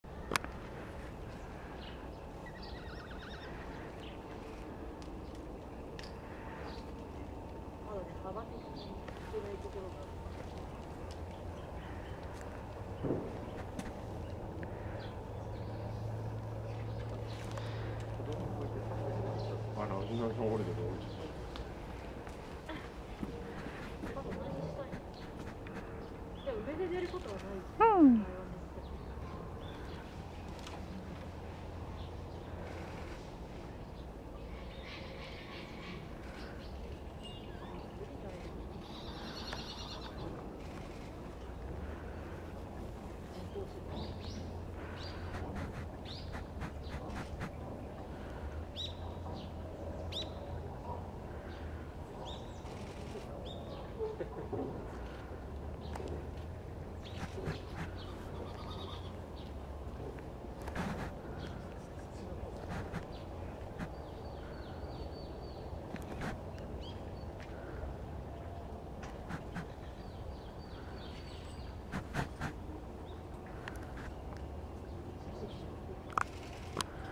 2021.12.6 10:09 釧路市動物園 レッサーパンダ 双子の赤ちゃん ① 2頭でまったり
アスナロちゃんとシンゲン子くんが🌳登り。
降りてきて、アスナロ子くんとまったり～な双子。
レンズのAFが思うようにいってくれなくて、ピントがズレたままでした😅